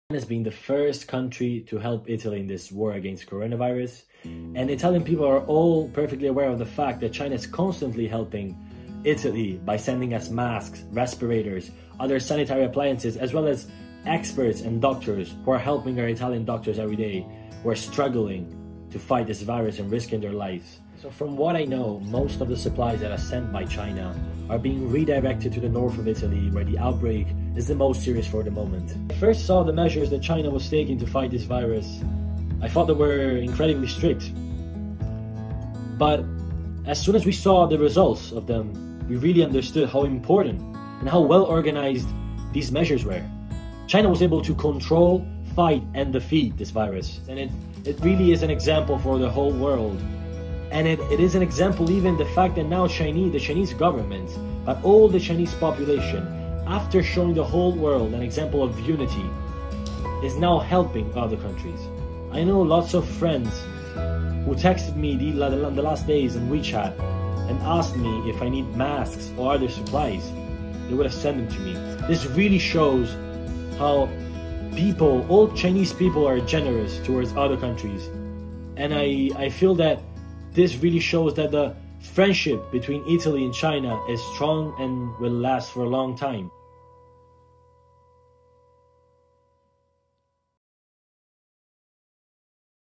Vlog: Italian student applauds China's help to his country
Are China's donations of medical supplies to Italy of bad quality? How is China helping the European country to fight COVID19? Nicholas Kovacich, an Italian student from Duke Kunshan University explains in his Vlog.